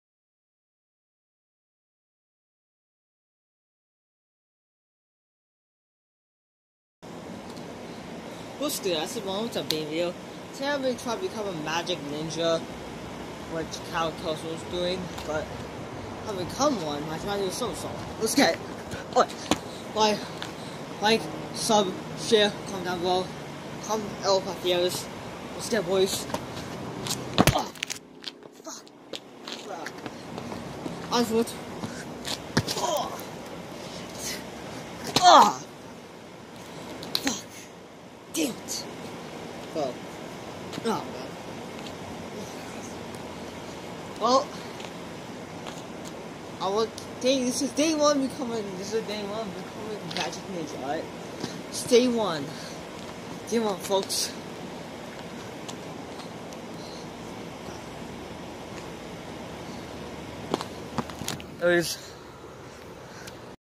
Day 1 of becoming a magic ninja (GONE WRONG) (OMG I FELL)
today bruh man tries to become a magic ninja by doing a somersault he will soon learn how to do cart wheels like, sub, and commet down below